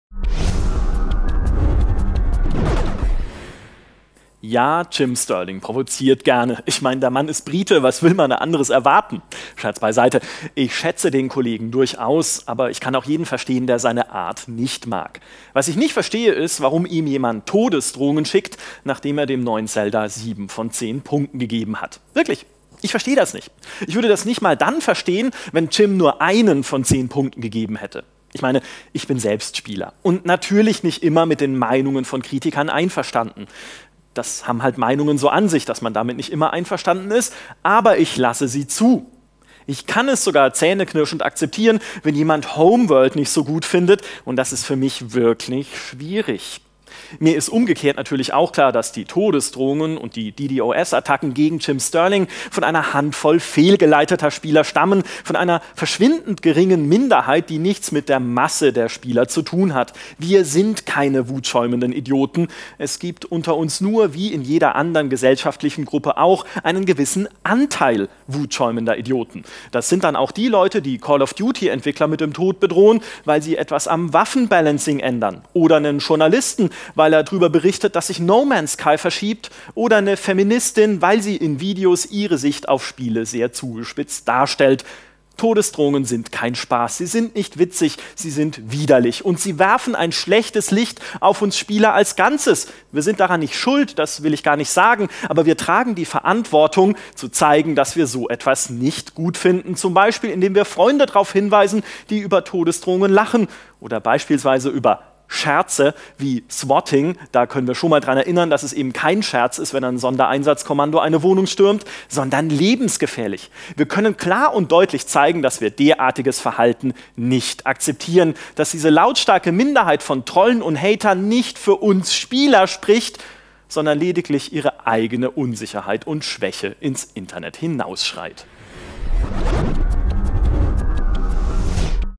Tötet den Kritiker! - Todesdrohungen gegen Zelda-Tester - Videokommentar
Der britische Spielekritiker Jim Sterling bekommt Todesdrohungen von wütenden Spielern, weil er The Legend of Zelda: Breath of the Wild »nur« 7 von 10 Punkten gegeben hat. Klar, könnte man da seufzen, das sind eben die üblichen Trolle und Hater, die sich geschmacklose Scherze erlauben. Stimmt ja auch. Doch Michael Graf findet: Das geht uns alle an! Denn wir dürfen nicht zulassen, dass laustarke Trolle und Hater bestimmen, wie die Öffentlichkeit uns Spieler wahrnimmt.

Niemand von uns kann und darf diesen Hass einfach tatenlos hinnehmen - egal, ob es um eine Zelda-Wertung geht oder um Todesdrohungen gegen einen Journalisten, der einfach nur darüber berichtet, dass No Man's Sky verschoben wurde. Oder ist das etwa der Umgangston, den wir uns beim schönsten Hobby der Welt wünschen?